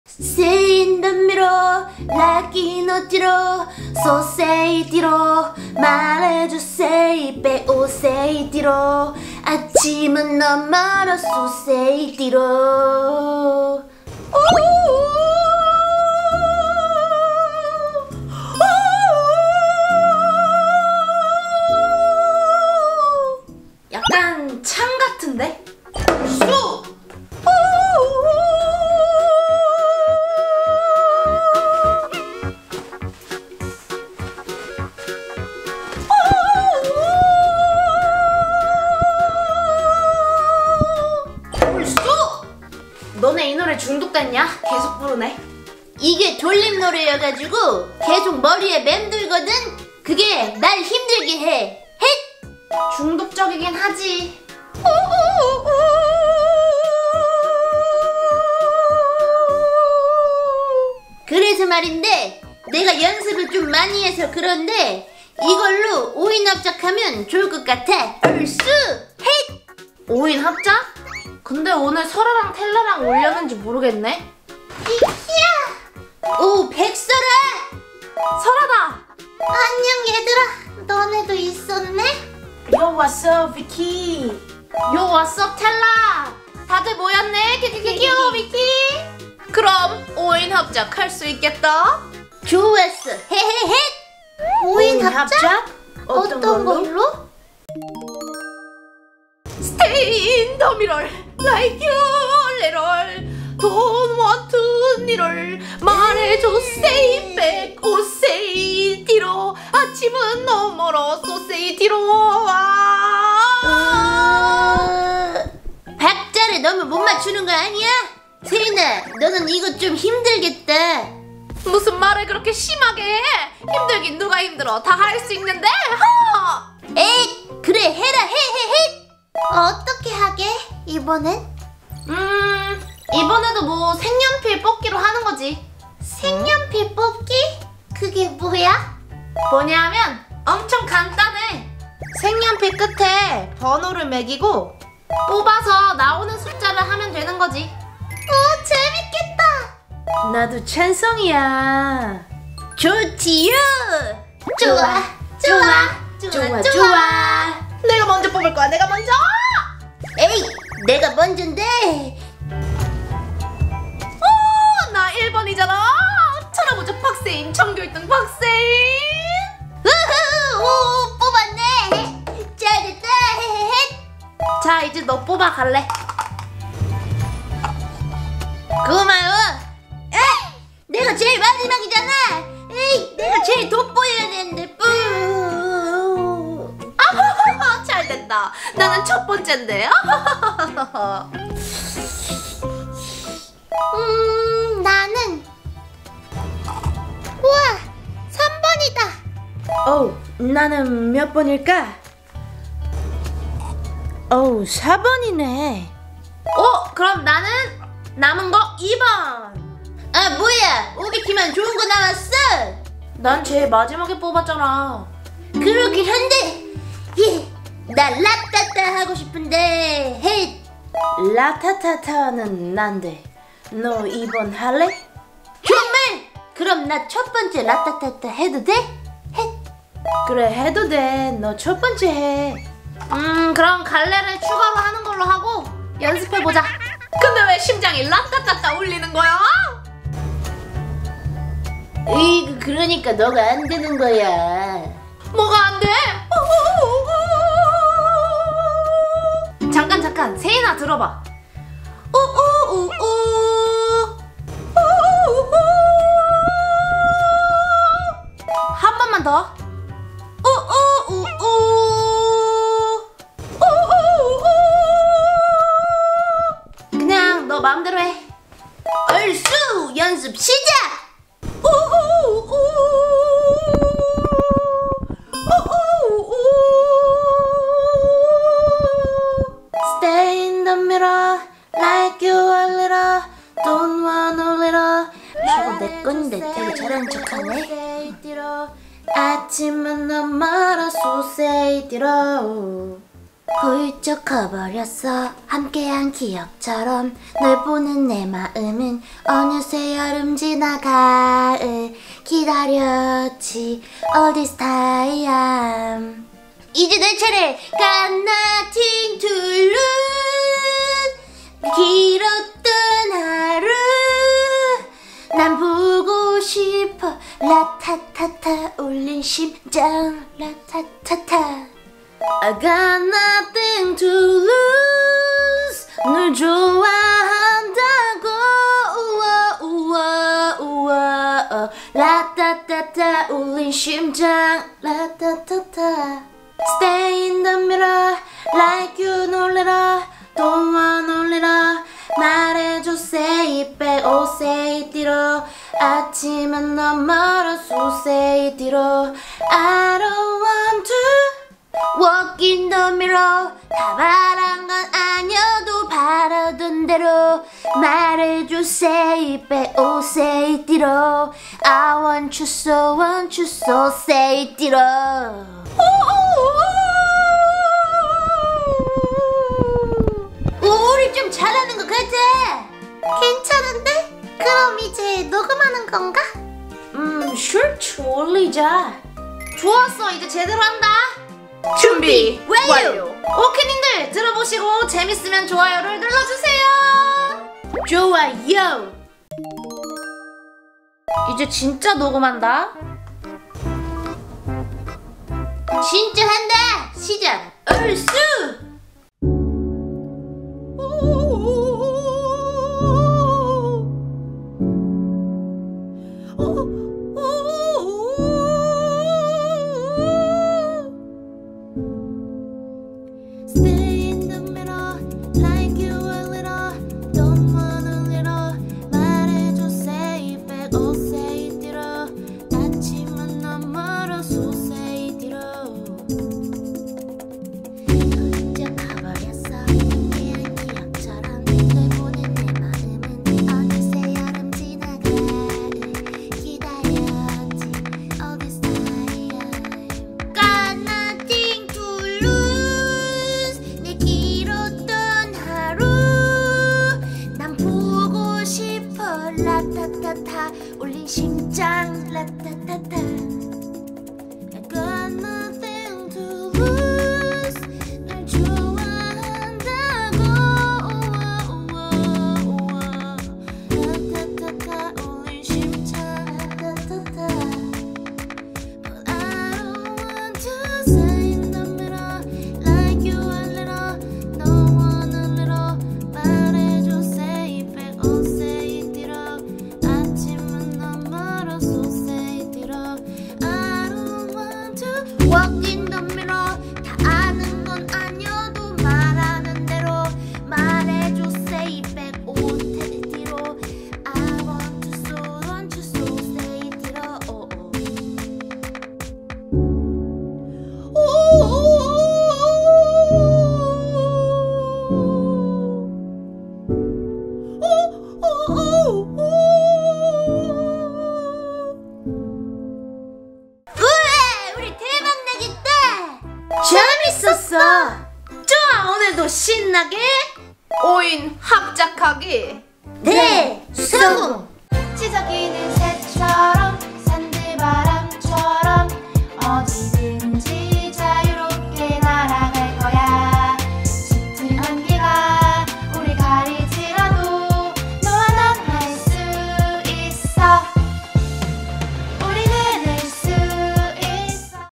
뉴진스 5인 합작
뉴진스 신곡에 빠진 양갈래와 친구들
합작을 하기로 하는데?
그 결과는?!